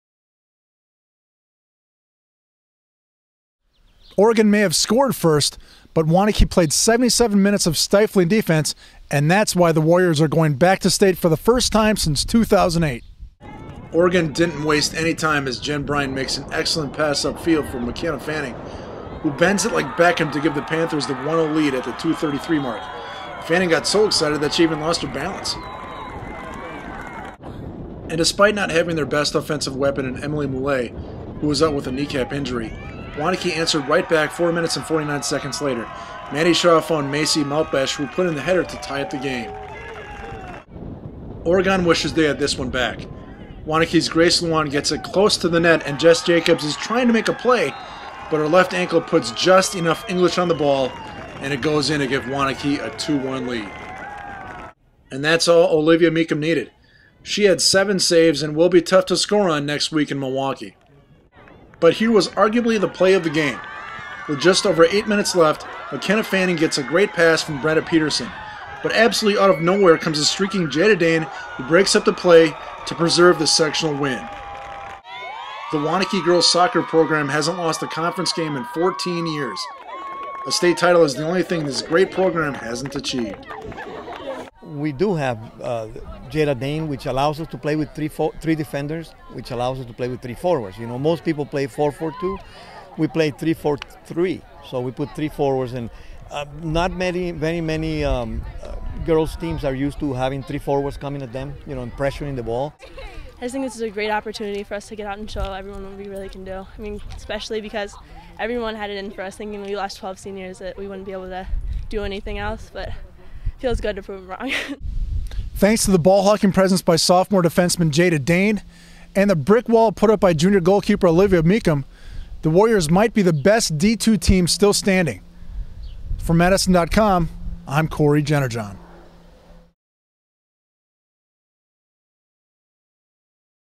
Stifling defense gives Waunakee state soccer berth
The Waunakee girls soccer team secured the second state berth in school history with a 2-1 win over Oregon in a WIAA Division 2 sectional final. Video/Cory Jennerjohn